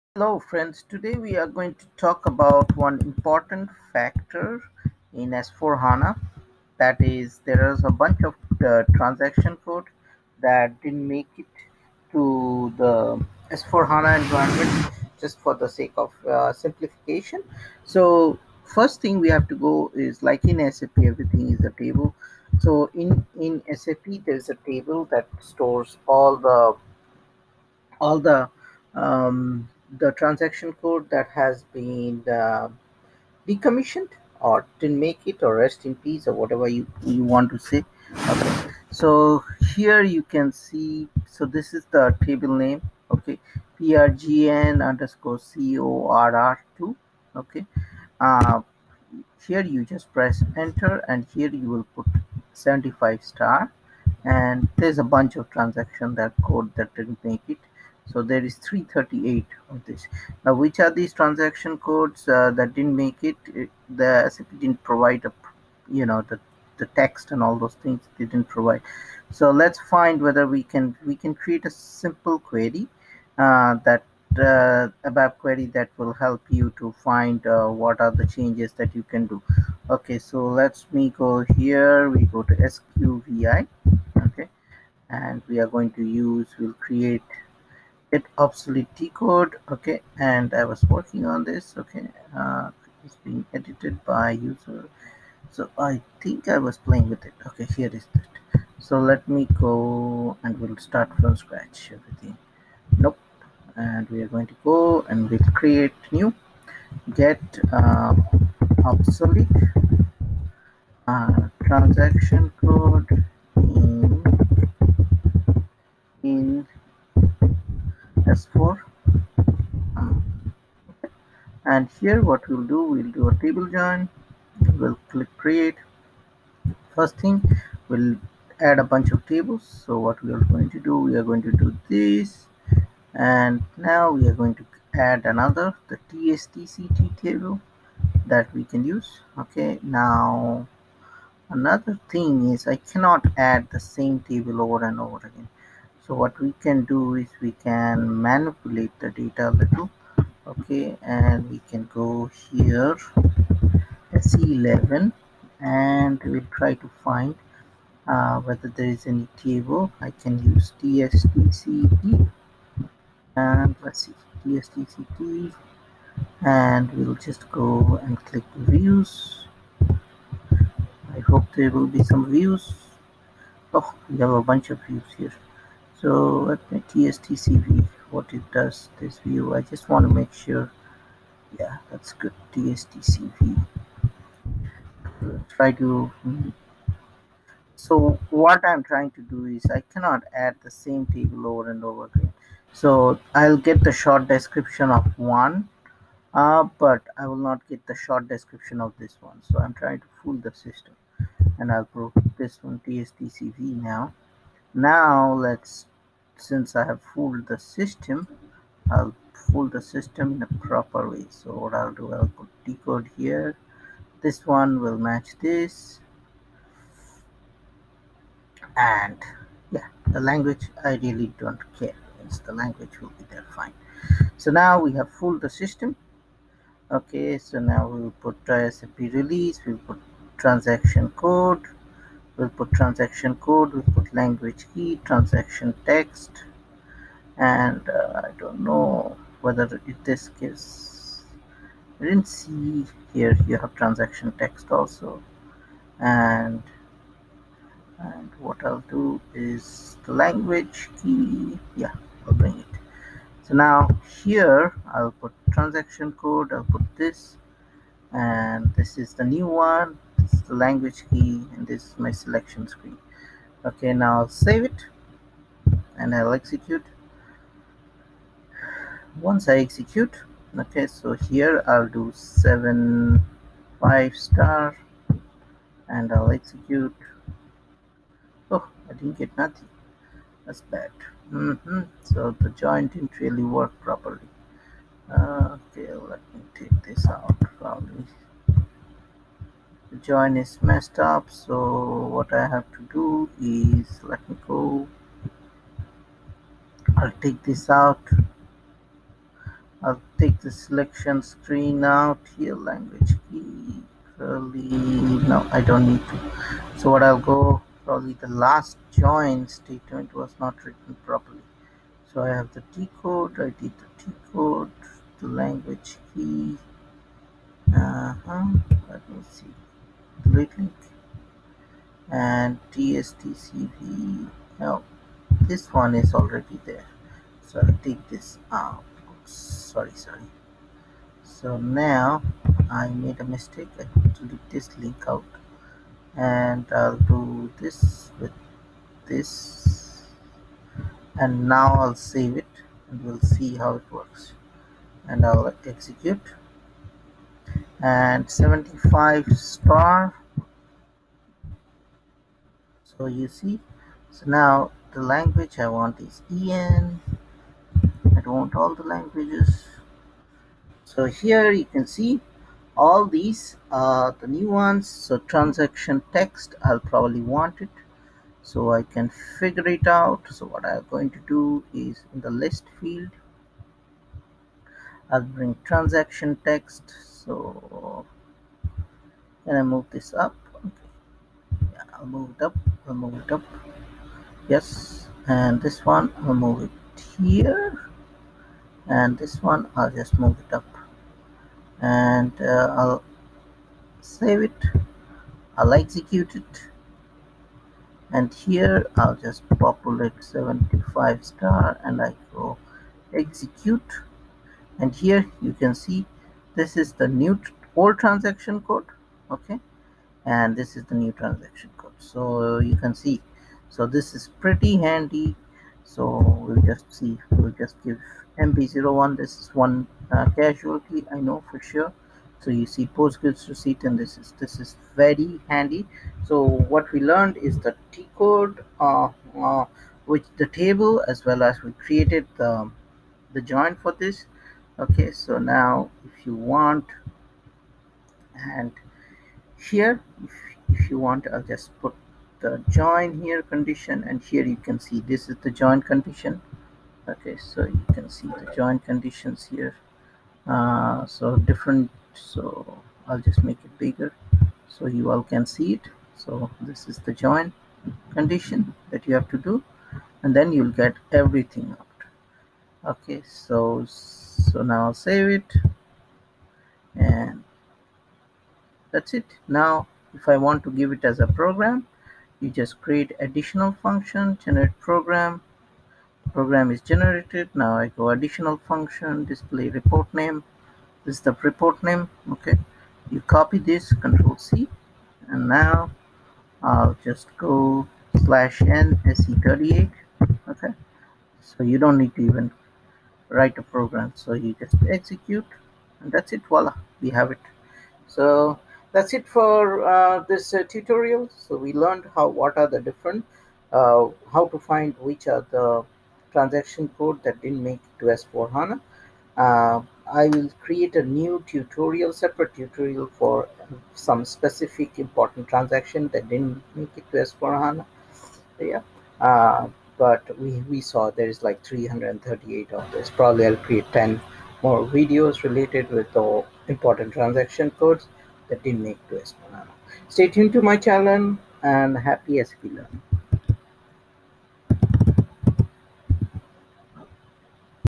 SAP ABAP: How to find which Transaction code that are Replaced by New Tcode in S/4HANA
How to find which Transaction code that are Replaced by New Tcode in S/4HANA